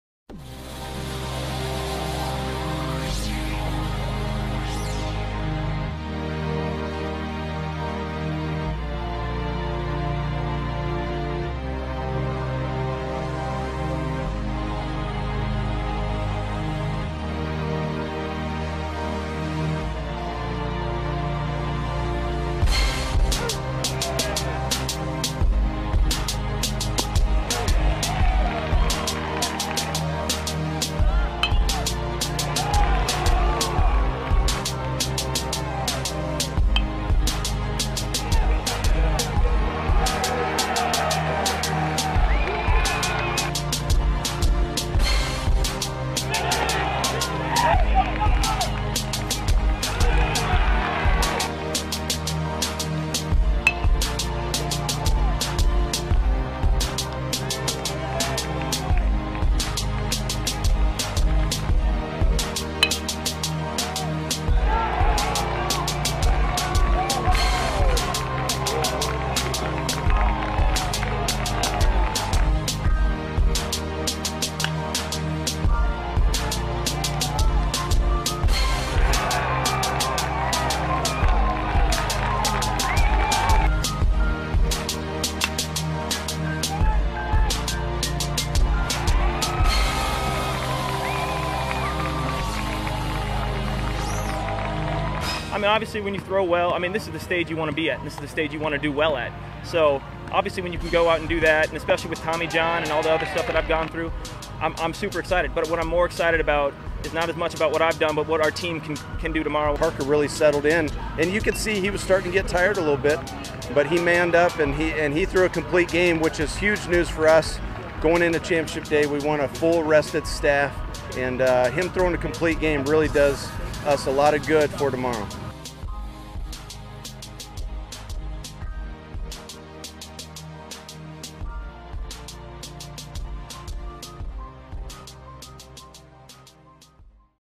NDSU Baseball Advances to Summit League Title Game for Third Straight Year
Behind a complete game effort from Parker Trewin, the Bison defeated Western Illinois 6-1 and advanced to the Summit League championship game tomorrow.